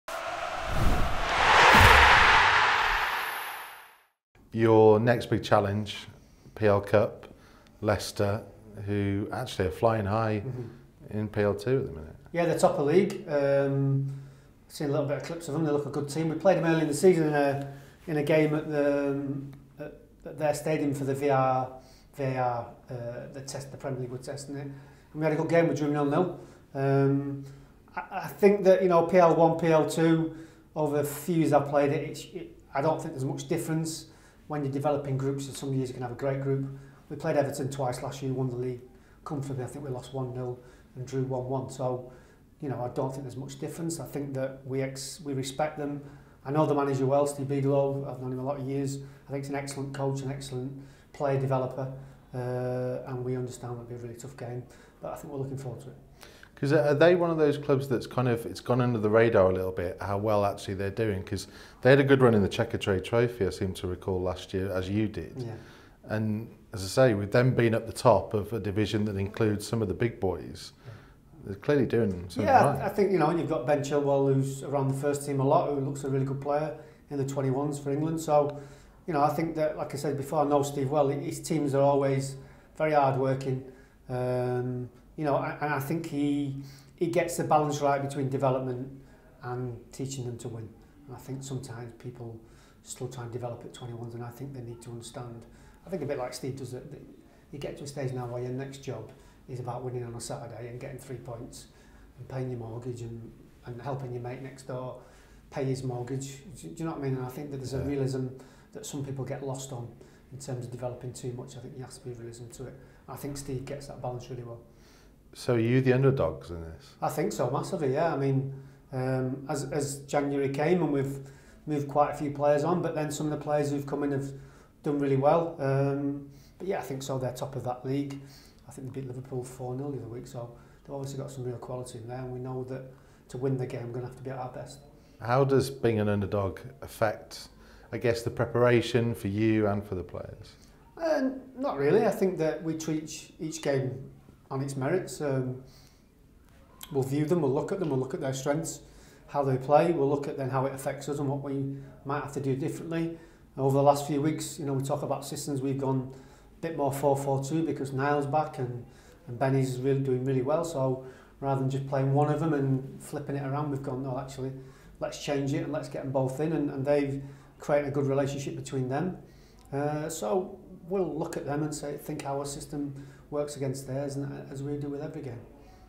Scott Sellars Speaks Ahead Of PL Cup Quarter Final
Scott Sellars speaks to Wolves TV ahead of Monday's Premier League Cup Quarter Final against Leicester City.